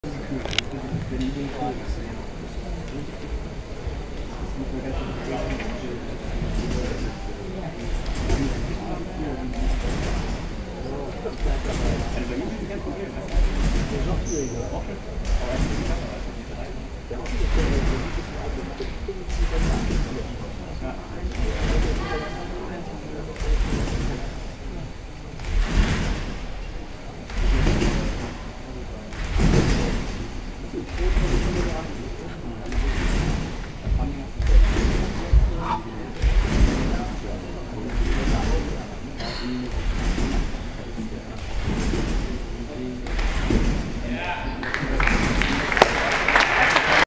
Dakota Earnest and Alaina Williams - Optional - 2011 Salzgitter World Cup
Score: 42.900 - Qualification - Sept. 2, 2011 - Salzgitter, Germany.